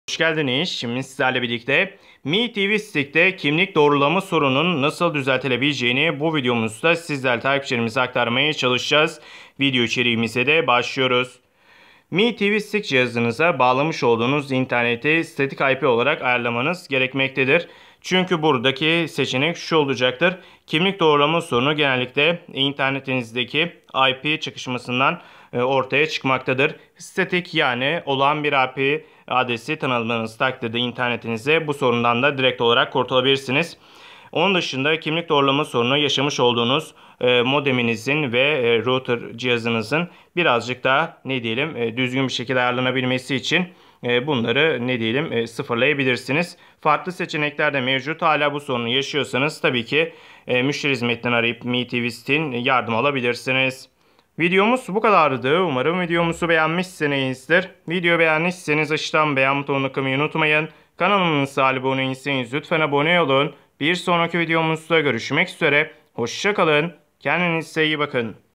Mi TV Stick Kimlik Doğrulama Sorunu Çözümü
Merhabalar bu videomuda sizlerle birlikte mi tv stick kimlik doğrulama nasıl yapılır sorusuna yanıt vermeye çalışıyoruz.İyi Seyiler Dilerim.

mi tv stick kimlik doğrulama ne demek
mi tv stick kimlik doğrulama hatası çözümü